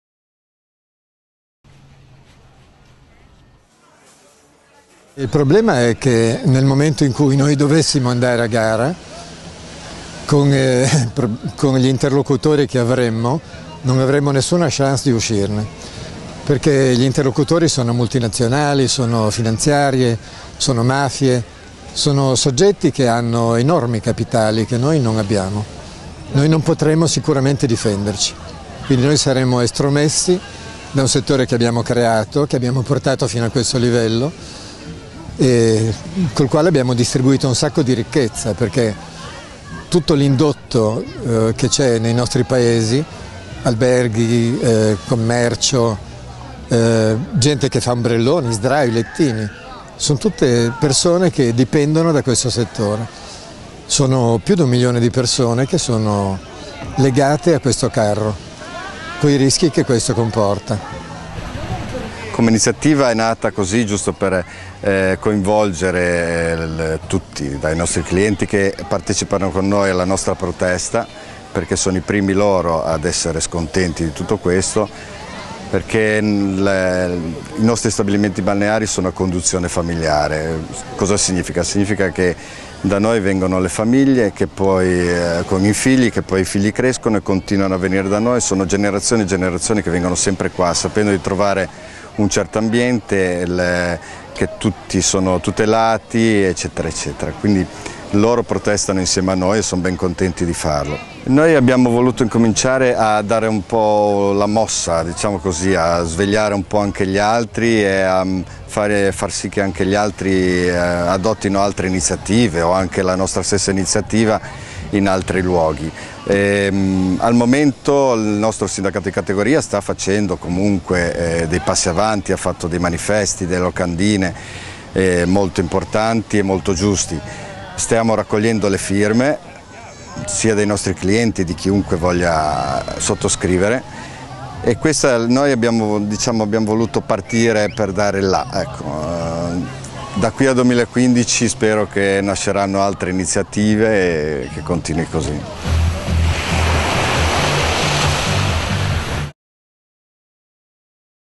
Lanterne volanti per protesta
La costa e il cielo sopra Varazze (Savona) si sono accesi la notte scorsa di lumini e di lanterne volanti, per dare "luce" alla protesta del Comitato Balneari Liguria contro gli effetti della direttiva europea Bolkestein.